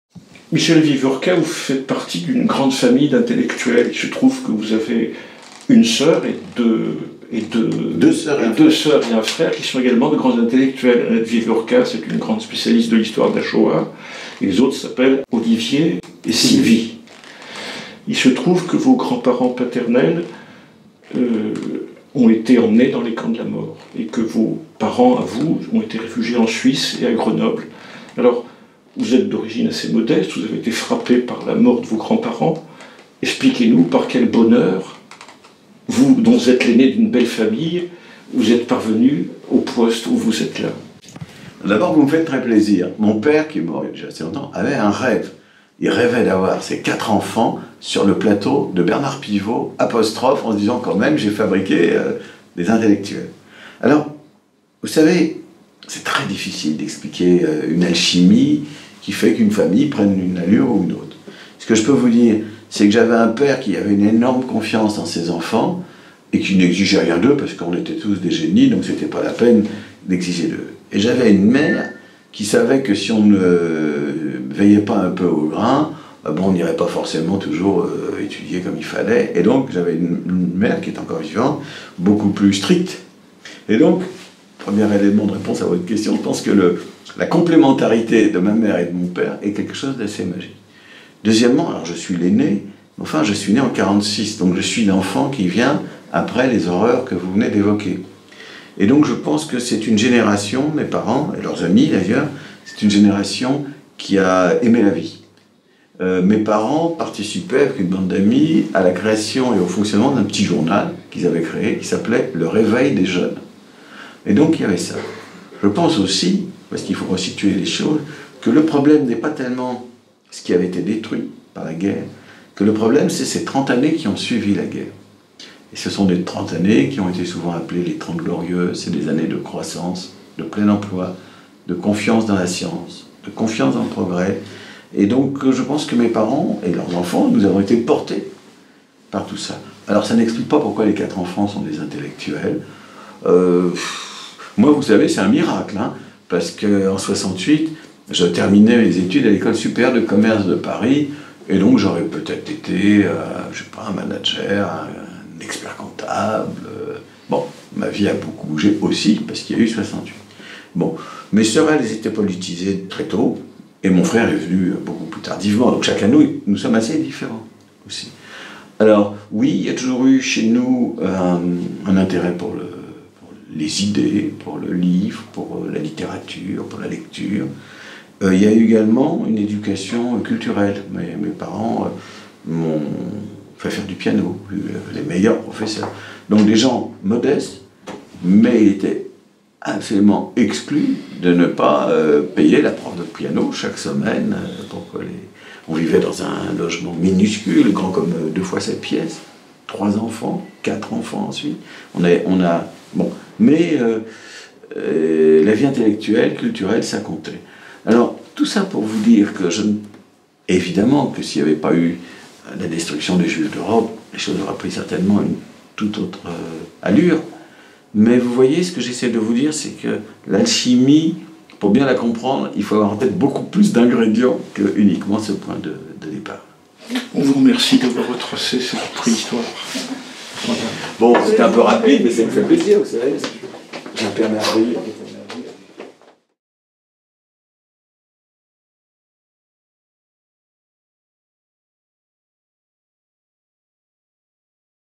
WebTV Zibeline - La question subsidiaire #19 - Avec Michel Wieviorka
Dans une loge du Mucem, quelques minutes avant son intervention du vendredi 13 avril, et à la suite de l'enregistrement d'un Grand Entretien de WRZ ( en écoute
Alain Paire invite le sociologue Michel Wieviorka à nous dire quelques mots sur sa famille.